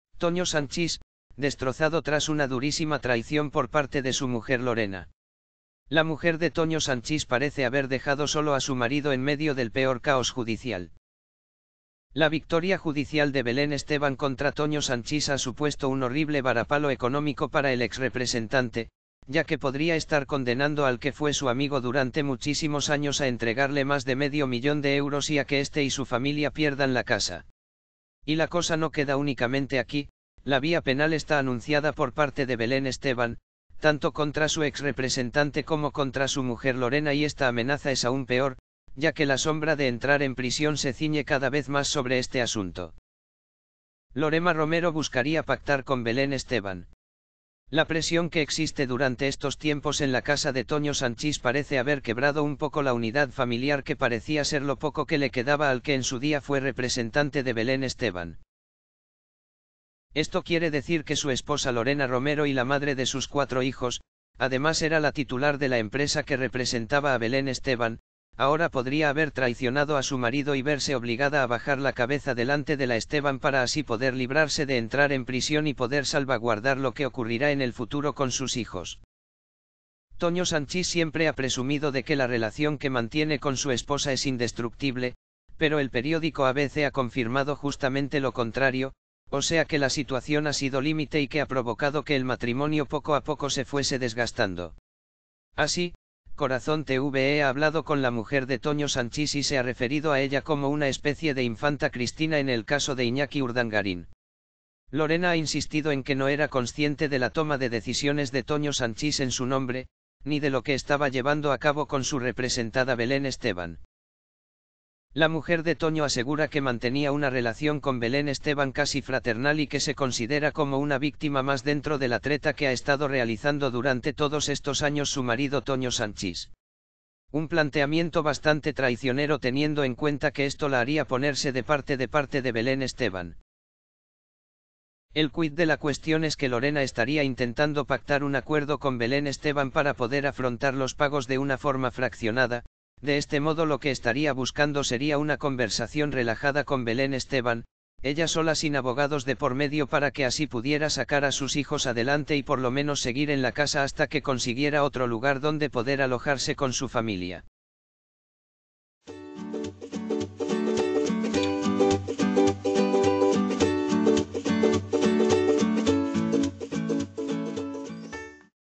Toño Sanchís, destrozado tras una durísima traición por parte de su mujer Lorena
★ Resumen ➜ La mujer de Toño Sanchís parece haber dejado solo a su marido en medio del peor caos judicial